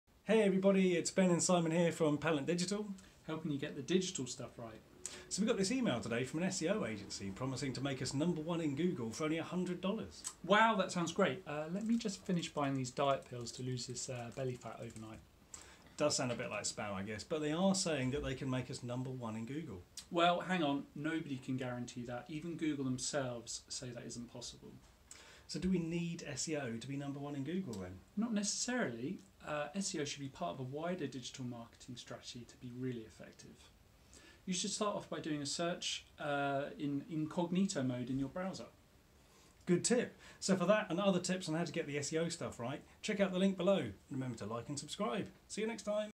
Make me No 1 in Google
Ben and Simon explain why you should be cautious of anyone who promises to make you number 1 in Google. For more great SEO tips check out the SEO blog